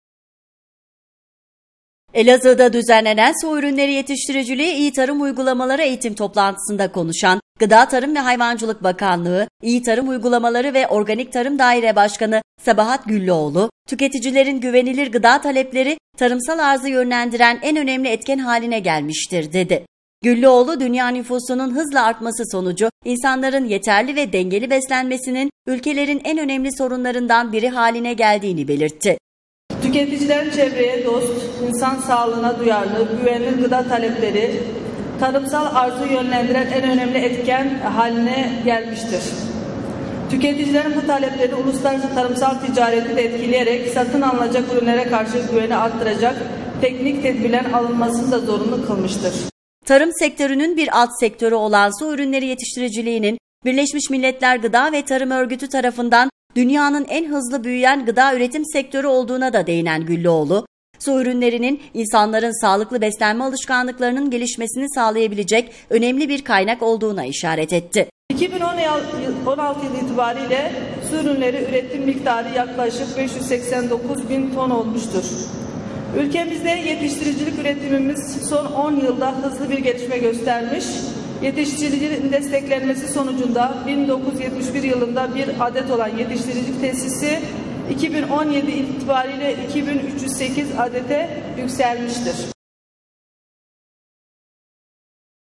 Sağlıklı beslenme için su ürünleri önemli bir kaynak
Su Ürünleri Yetiştiriciliği İyi Tarım Uygulamaları Eğitim Toplantısı Elazığ’da yapıldı.
Sağlıklı beslenme için su ürünleri önemli bir kaynak Elazığ’da düzenlenen Su Ürünleri Yetiştiriciliği İyi Tarım Uygulamaları Eğitim Toplantısı’nda konuşan Gıda Tarım ve Hayvancılık Bakanlığı İyi Tarım Uygulamaları ve Organik Tarım Daire Başkanı Sabahat Güllüoğlu, "Tüketicilerin güvenilir gıda talepleri tarımsal arzı yönlendiren en önemli etken haline gelmiştir." dedi.

Güllüoğlu, dünya nüfusunun hızla artması sonucu insanların yeterli ve dengeli beslenmesinin ülkelerin en önemli sorunlarından biri haline geldiğini belirtti.

Bu sorunun çözümünde, mevcut kaynakların daha verimli kullanılmasının yanında alternatif gıda kaynakları ve üretim teknikleriyle ilgili çalışmalara da hız verildiğini ifade eden Güllüoğlu, şöyle konuştu:

"Tüketicilerin artan gıda ihtiyaçlarını karşılamak ve üreticilerin yaşam seviyelerini yükseltmek amacıyla birim alandan elde edilen ürünü artırmaya dayalı olarak çevreye zarar vermeyen, güvenli gıda üretimini sürdürebilmeyi sağlayan tarımsal politikalar gündeme gelmeye başlamıştır. Tüketicilerin çevreye dost, insan sağlığına duyarlı, güvenilir gıda talepleri tarımsal arzı yönlendiren en önemli etken haline gelmiştir. Tüketicilerin bu talepleri tarımsal ticareti de etkileyerek satın alınacak ürünlere karşı güveni artıracak teknik tedbirlerin alınmasını da zorunlu kılmıştır."

Güllüoğlu, küresel ölçekte gıda güvenliği sorunlarının çözümünde ekonomik açıdan karlı, sosyal açıdan çevreye zarar vermeyen ve sürdürülebilir kalkınmayı sağlayan politikaların gelişmeye başladığını dile getirdi.
Bu noktada ülkede iyi tarım uygulamalarının başlatıldığını aktaran Güllüoğlu, artan talebin iyi tarım uygulamasını 10 yıllık süreçte yaklaşık 115 kat artırdığını ifade etti.

Gülloğlu, "İyi tarım uygulamaları ülkemizde 2007'de 18 ilde 651 üretici ve 53 bin 607 dekar alanda yapılırken 2017'ye geldiğimizde 64 ilde 72 bin 236 üretici ve 6 milyon 247 bin dekar alanda yapılır hale gelmiştir." dedi.

Sağlıklı beslenmede en önemli kaynak su ürünleri

Tarım sektörünün bir alt sektörü olan su ürünleri yetiştiriciliğinin Birleşmiş Milletler Gıda ve Tarım Örgütü (FAO) tarafından dünyanın en hızlı büyüyen gıda üretim sektörü olduğuna da değinen Güllüoğlu, su ürünlerinin insanların sağlıklı beslenme alışkanlıklarının gelişmesini sağlayabilecek önemli bir kaynak olduğuna işaret etti.

Türkiye'de 2016'da toplam su ürünleri arzının yüzde 57'sinin avcılık ve yüzde 43'ünün yetiştiricilik yoluyla elde edildiğini aktaran Güllüoğlu, şunları kaydetti:

"2016 yılı itibarıyla su ürünleri üretim miktarı yaklaşık 589 bin ton olmuştur. Ülkede yetiştiricilik üretimimiz son 10 yılda hızlı bir büyüme göstermiş, yetiştiriciliğin desteklenmesi sonucunda 1971 yılında 1 olan yetiştiricilik tesisi 2017 itibarıyla 2 bin 308'e yükselmiştir. Su ürünleri yetiştiriciliği iyi tarım uygulamalarında ise 2012 yılında 3 işletme 8 bin 320 ton ile başlamış olup 2017 yılında desteklenme kapsamına alınmasıyla çipura, levrek, alabalık ve granyöz üretimi yapan 54 işletmede 71 bin 217 tona ulaşılmıştır."